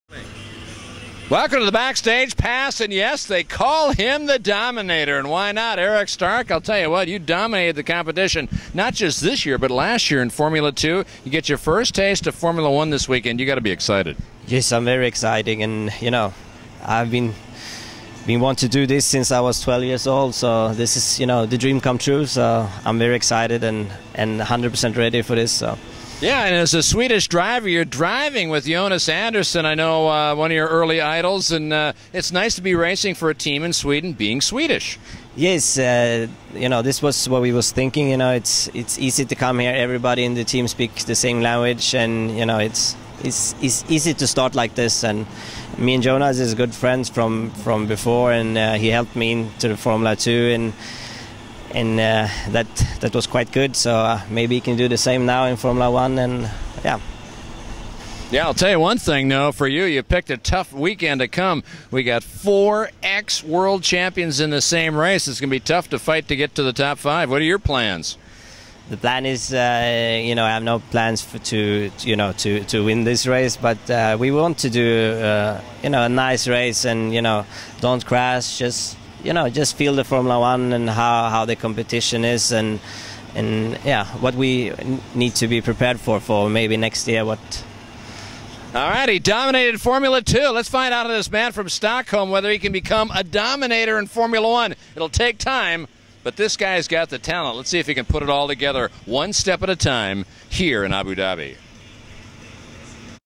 Erik Stark "The Dominator"
Abu Dhabi, November 28 - Interview of Erik Stark from Sweden, Team Sweden at his first F1 Grand Prix.